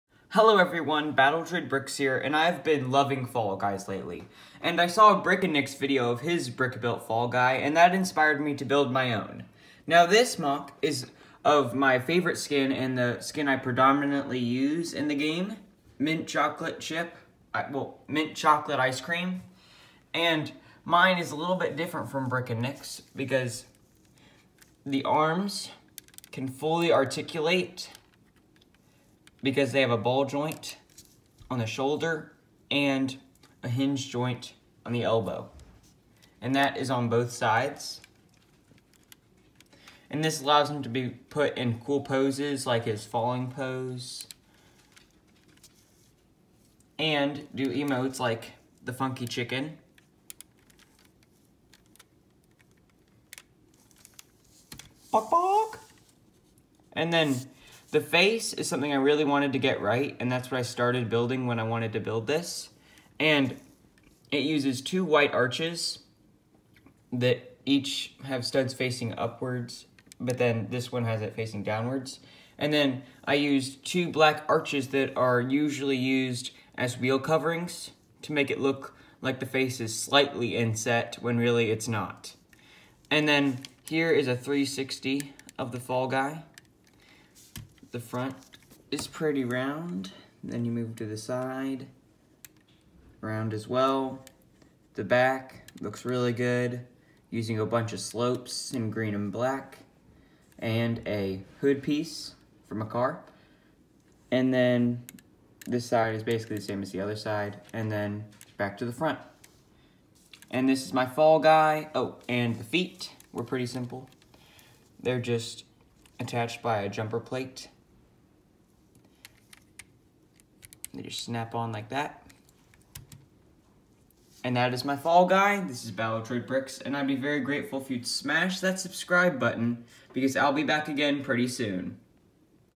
LEGO Fall Guys (Mint Chocolate Ice Cream Skin)
In this video, I show you my moc of the Mint Chocolate Ice Cream skin from Fall Guys. This is my favorite skin and the one I predominantly use.